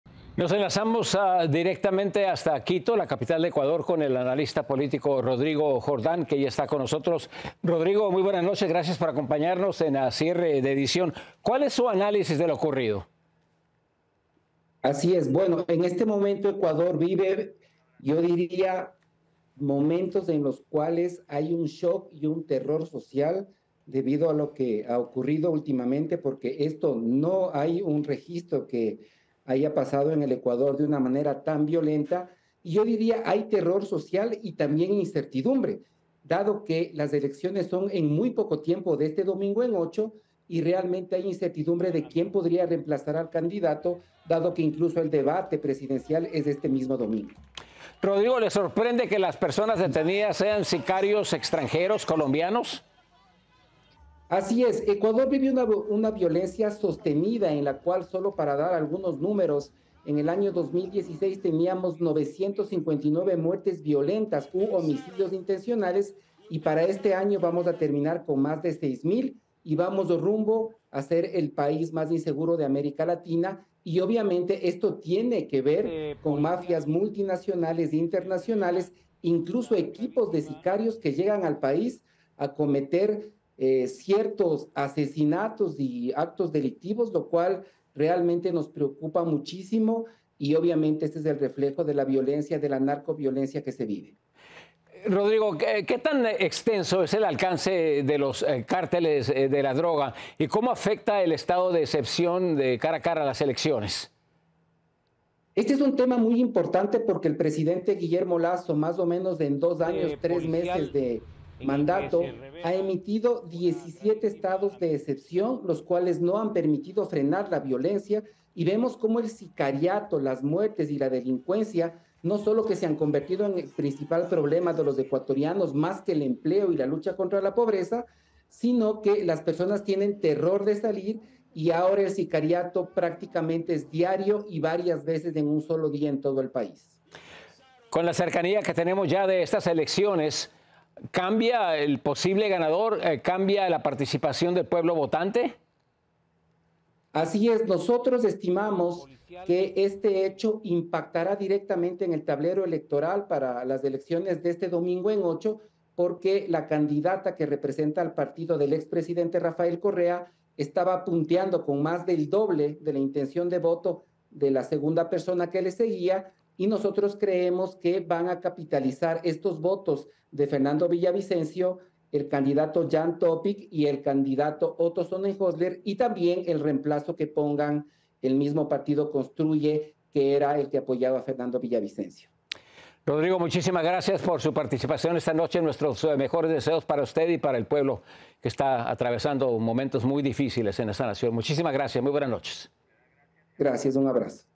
Crisis de inseguridad en Ecuador | Entrevista con Rodrigo Jordán
Entrevista | 🇪🇨 Desde Ecuador, el analista político Rodrigo Jordán nos habla sobre la difícil situación de seguridad que vive el país tras el asesinato del candidato presidencial Fernando Villavicencio.